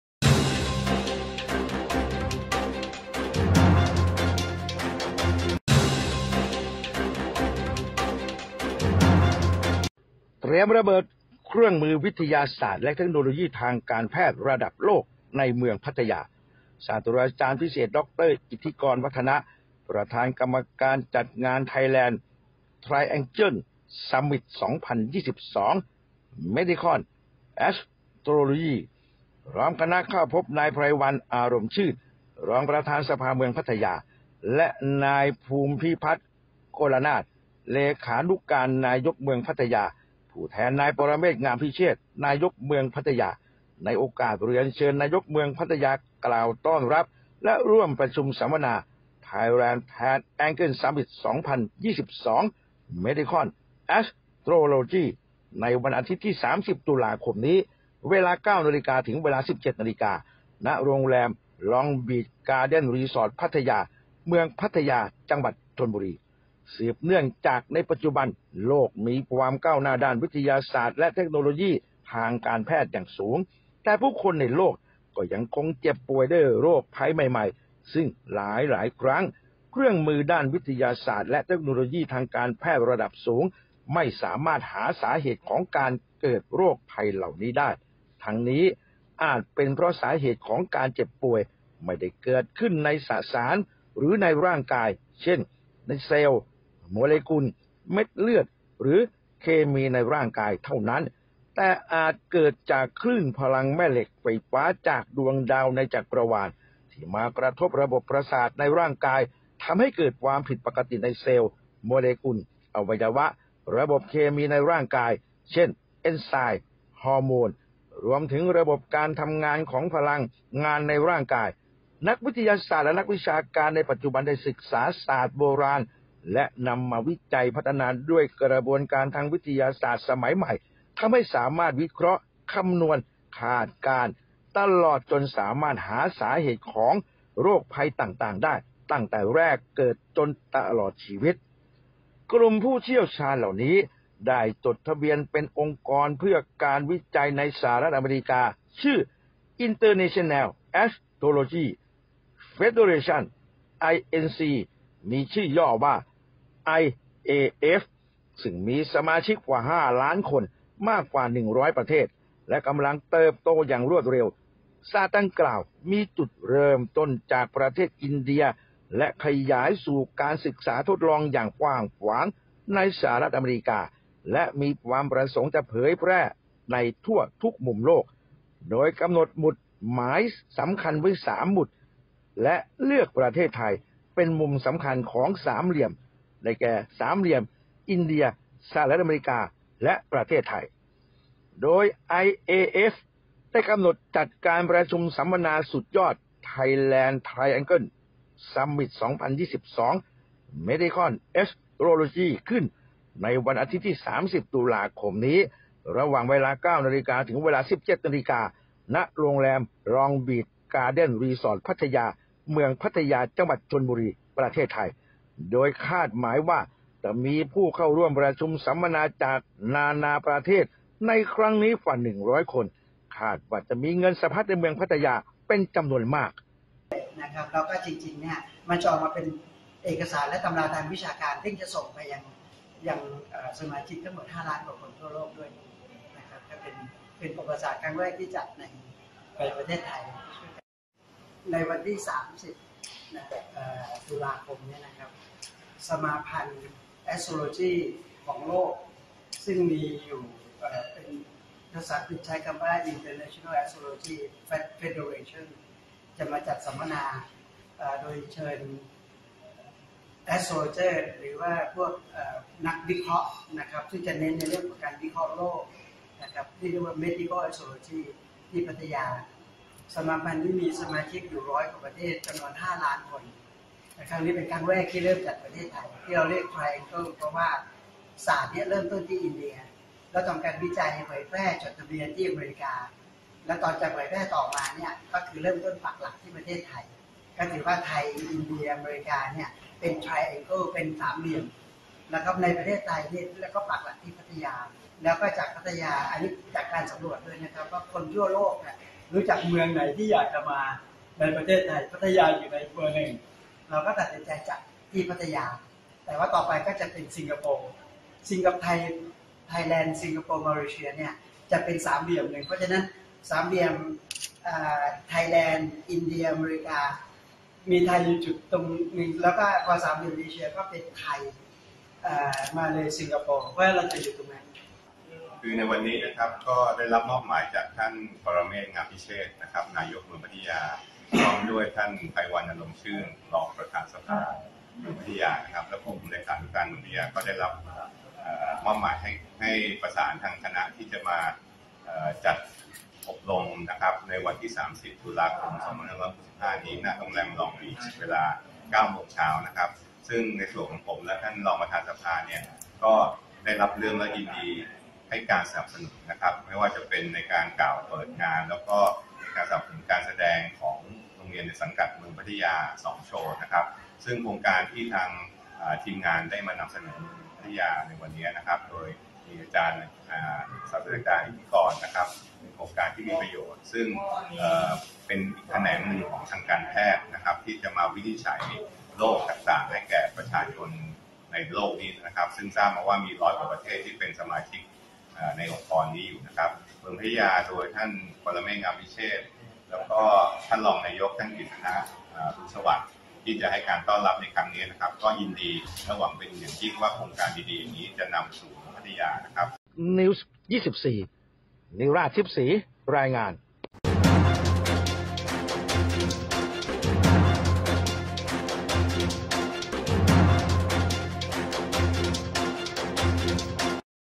เตรียมระเบิด เครื่องมือวิทยา ศาสตร์และเทคโนโลยีทางการแพทย์ระดับโลก ในเมืองพัทยา
ศาสตราจารย์พิเศษ ดร.อิทธิกร วัฒนะ ประธานกรรมการจัดงาน Thailand Triangle Summit 2022 : Medical Astrology พร้อมคณะเข้าพบ นายไพรวัลย์ อารมณ์ชื่น รองประธานสภาเมืองพัทยา และนายภูมิพิพัฒน์ กลนาถ เลขานุการนายกเมืองพัทยา ผู้แทนนาย ปรเมศวร์ งามพิเชษฐ์ นายกเมืองพัทยา ในโอกาสเรียนเชิญนายกเมืองพัทยา กล่าวต้อนรับและร่วมประชุมสัมนา  Thailand Triangle Summit 2022 : Medical Astrology ในวันอาทิตย์ ที่ 30 ตุลาคม 2565 เวลา 9.00 ถึง 17.00 น ณ โรงแรม Long Beach Garden Resort, Pattaya เมืองพัทยา จ.ชลบุรี
           สืบเนื่องจาก ปัจจุบันโลกมีความก้าวหน้าด้านวิทยาศาสตร์และเทคโนโลยีทางการแพทย์อย่างสูง แต่ผู้คนในโลกก็ยังคงเจ็บป่วยด้วยโรคภัยใหม่ๆ ซึ่งหลายๆ ครั้งเครื่องมือด้านวิทยาศาสตร์และเทคโนโลยีทางการแพทย์ระดับสูง ไม่สามารถหาสาเหตุการเกิดโรคภัยเหล่านี้ได้ ทั้งนี้อาจเป็นเพราะสาเหตุของการเจ็บป่วยไม่ได้เกิดขึ้นในสสารหรือในร่างกาย เช่น เซลล์ โมเลกุล เม็ดเลือด หรือเคมี ในร่างกายเท่านั้น แต่อาจกิดจากคลื่นพลังงานแม่เหล็กไฟฟ้าจากดวงดาวในจักรวาล มากระทบระบบประสาทในร่างกาย ทำให้เกิดความผิดปกติในเซลล์ โมเลกุล อวัยวะ ระบบเคมีในร่างกาย เช่น เอนไซม์ ฮอร์โมน รวมถึงระบบการทำงานของพลังานในร่างกาย นักวิทยาศาสตร์และนักวิชาการปัจจุบันได้ศึกษาศาสตร์โบราณและนำมาวิจัยพัฒนาด้วยกระบวนการทางวิทยาศาสตร์ สมัยใหม่ ทำให้สามารถวิเคราะห์คำนวณคาดการณ์ ตลอดจนสามารถหาสาเหตุโรคภัยต่าง ๆ ได้ตั้งแต่แรกเกิดจนตลอดชีวิต
          กลุ่มผู้เชี่ยวชาญเหล่านี้ใด้จดทะเบียนเป็นองค์กร เพื่อการวิจัยในสหรัฐอเมริกาชื่อ International Astrology Federation Inc (IAF) มีสมาชิกกว่า 5 ล้านคน มากกว่า 100 ประเทศ และกำลังเติบโตอย่างรวดเร็ว ศาสตร์ดังกล่าวมีจุดเริ่มต้นจาก ประเทศอินเดียและขยายสู่การศึกษาทดลองอย่างกว้างขวาง ในสหรัฐอเมริกา และมีความประสงค์จะเผยแพร่ในทั่วทุกมุมโลก โดยกำหนดหมุดหมายสำคัญ 3 มุม (Triangle) และเลือกประเทศไทย เป็นมุมสำคัญของสามเหลี่ยม อินเดีย สหรัฐอเมริกาและประเทศไทย
         โดย IAF กำหนดจัดการประชุมสัมมนาสุดยอด Thailand Triangle Summit 2022 : Medical Astrology ขึ้นในวันอาทิตย์ที่ 30 ตุลาคม 2565 ระหว่างเวลา 9.00 ถึง 17.00 น ณ โรงแรม Long Beach Garden Resort, Pattaya เมืองพัทยา จ.ชลบุรี ประเทศไทย โดยคาดหมายผู้เข้าร่วมประชุมสัมนาจากนานาประเทศ ในครั้งนี้กว่า 100 คน คาดว่าจะมีเงินสะพัดในเมืองพัทยา จำนวนมาก
นิราช ทิพย์ศรี /นันทพล  ทิพย์ศรี  อ.สัตหีบ จ.ชลบุรี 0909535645,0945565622/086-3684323